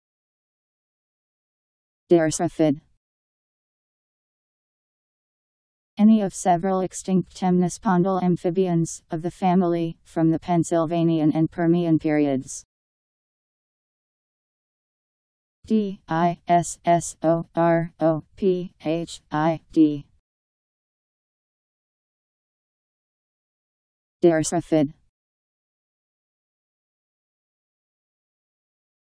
Dissorophid Meaning
Video shows what dissorophid means. Any of several extinct temnospondyl amphibians, of the family , from the Pennsylvanian and Permian periods.  Dissorophid Meaning. How to pronounce, definition audio dictionary. How to say dissorophid. Powered by MaryTTS, Wiktionary